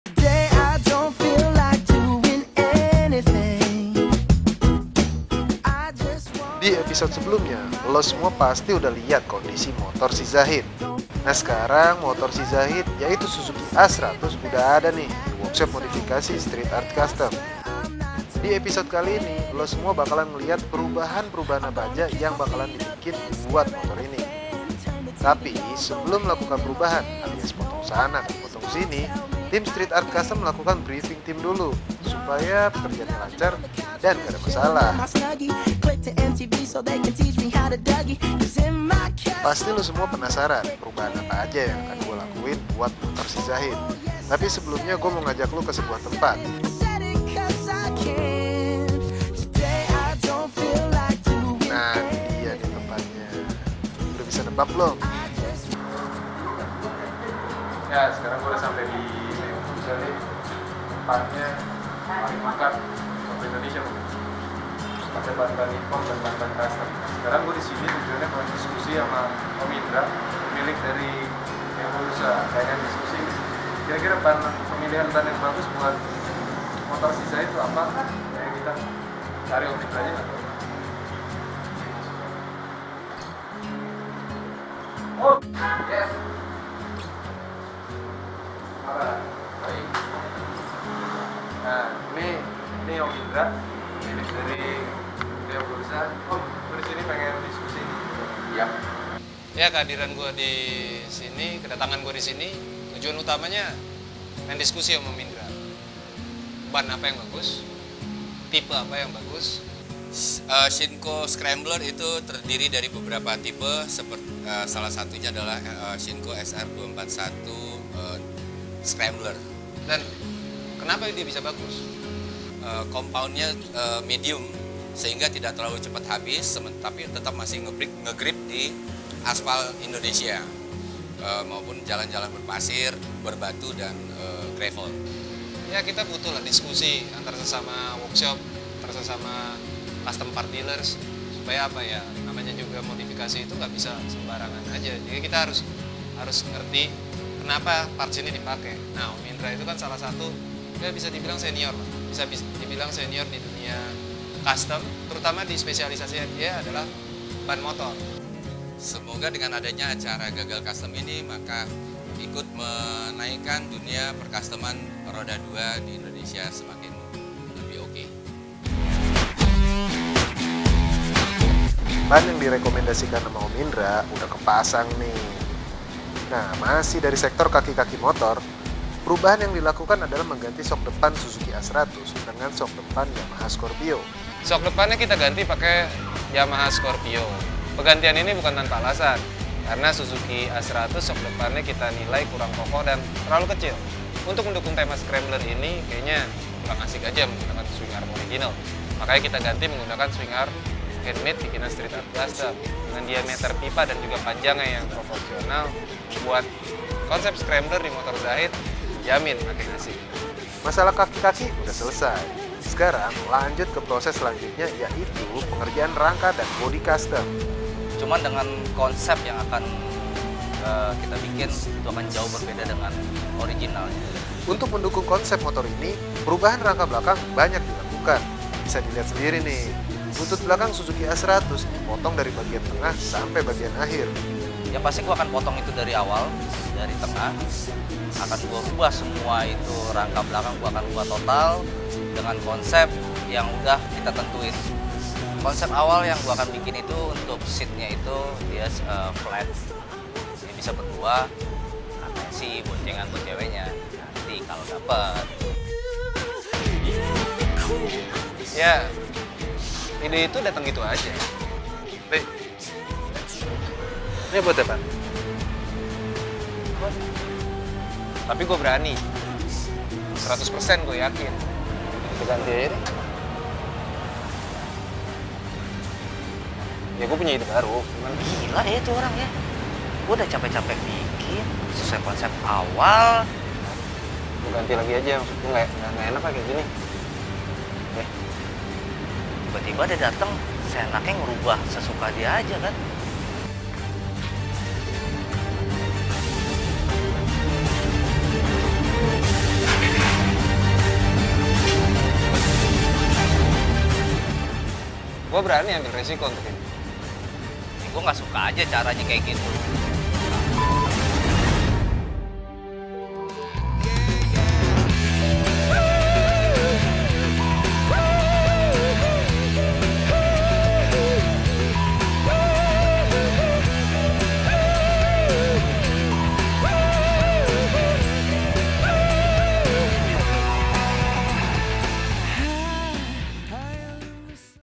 Gagalkustom eps 2
GAGALKUSTOM adalah program yang dibuat untuk mensupport acara evet PARJO BANDUNG pada tahun 2016, dimana hasil motor ini hanya diperlihatkan di event parjo bandung...
namun foto hasil modifikasi motor ini bisa dilihat di instagram @streetartscustom